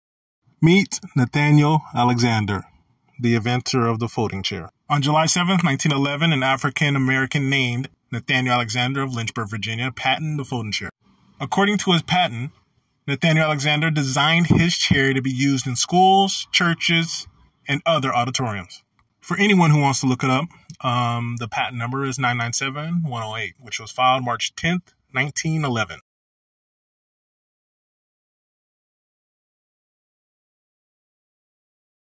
Inventor Of The Folding Chair
Nathaniel Alexander the inventor of the folding chair.

*the info is correct but the picture might be wrong. Nathaniel Alexander is a black man who looks similar.

*this may be a picture of Vladimir who invented the working light bulb